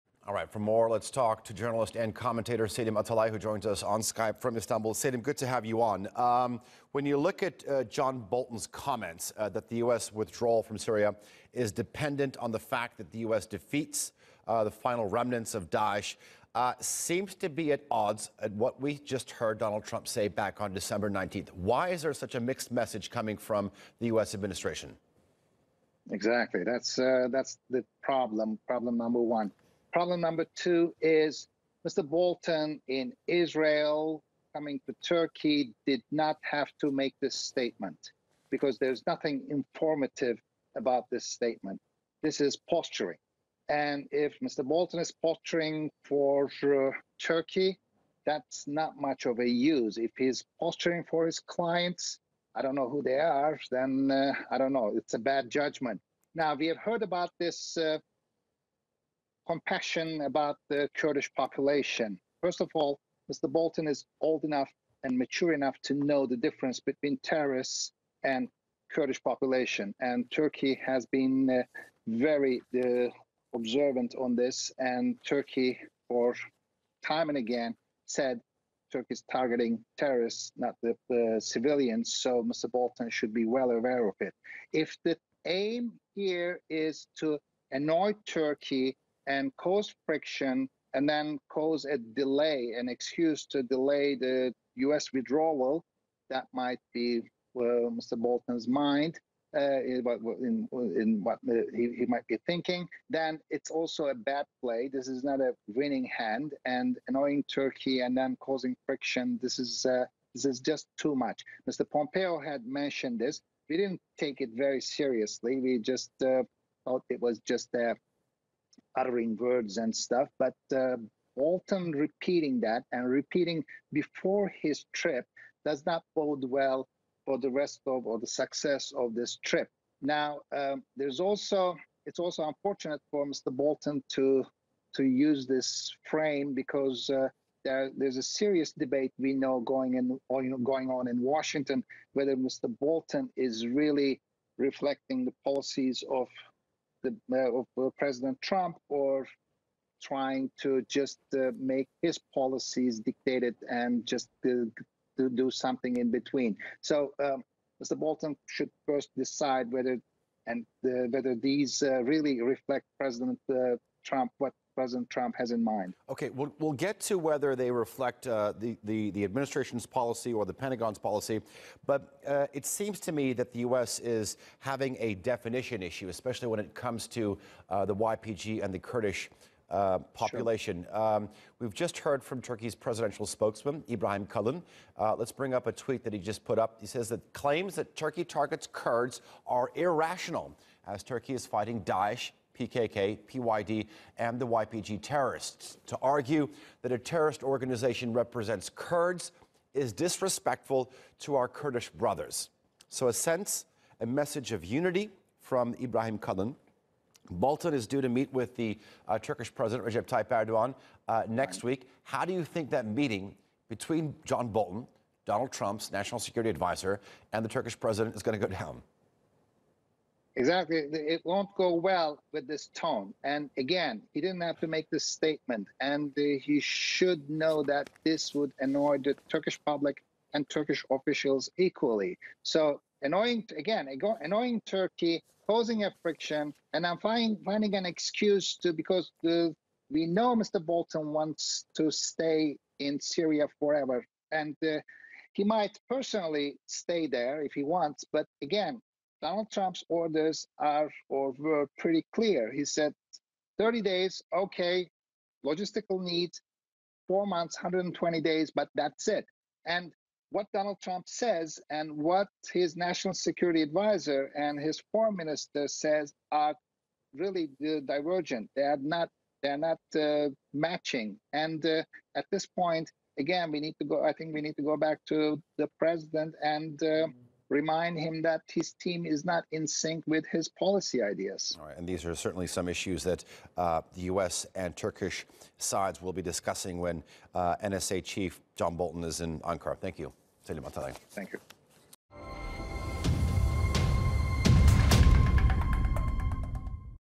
The War in Syria: Interview with Selim Atalay
Selim Atalay, Journalist and Commentator, is talking about   the war in Syria and Bolton's comments.